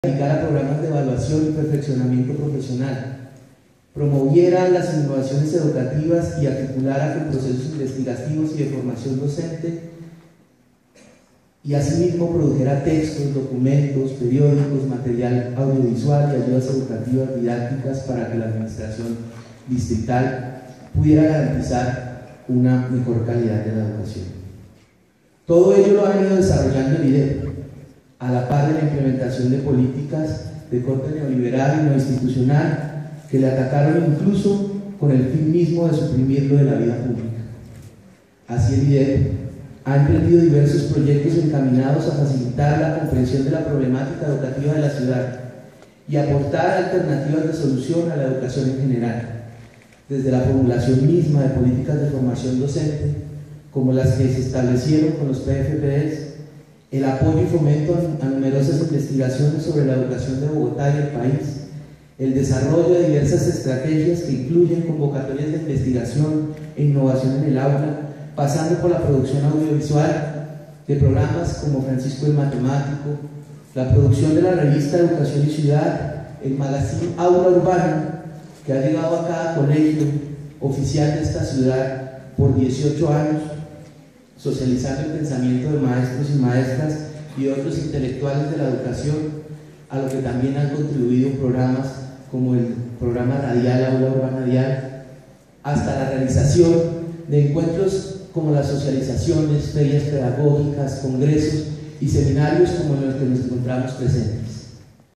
Paulo Molina, sub director IDEP
El subdirector del IDEP, Paulo Molina, presenta saludo de bienvenida a los asistentes del Seminario Latinoamericano La Escuela Hoy.